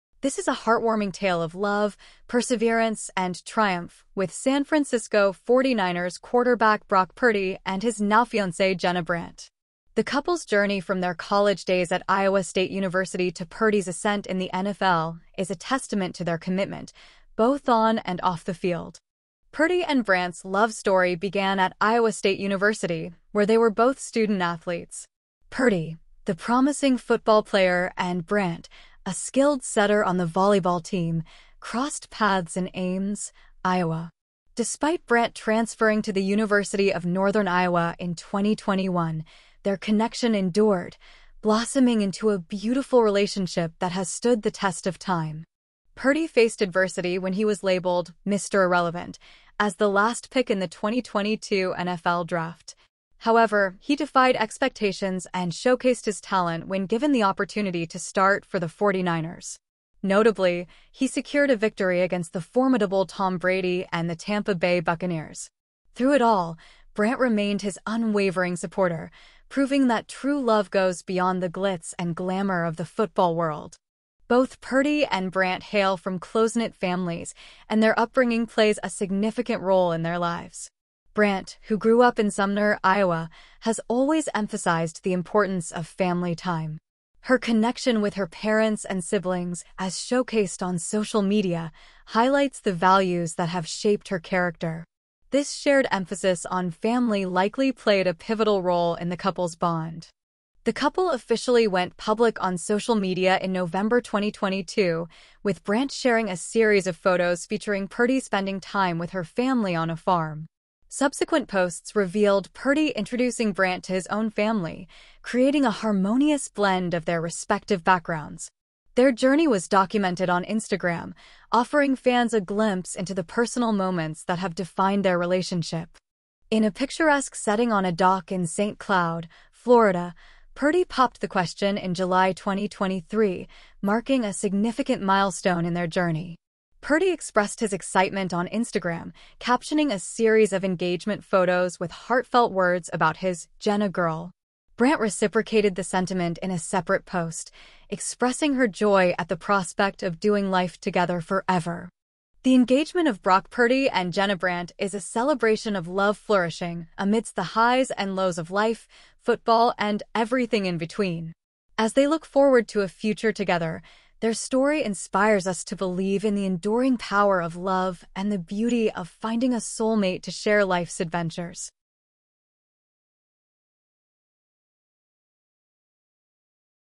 Quarterback Brock Purdy and Jenna Brandt's University Love Story
Quarterback Brock Purdy and Jenna Brandt's love story began on the vibrant campus of Iowa State University, where their paths intertwined amidst the excitement of university life. From the cheering crowds of football games to the camaraderie of campus events, their connection deepened as they navigated the challenges of college together. As Purdy honed his skills on the field, Brandt, a former volleyball standout, provided unwavering support. Their university love story, rooted in shared passions for both each other and the game, has since blossomed into a beautiful journey that has stood the test of time, culminating in a recent engagement that reflects their enduring bond.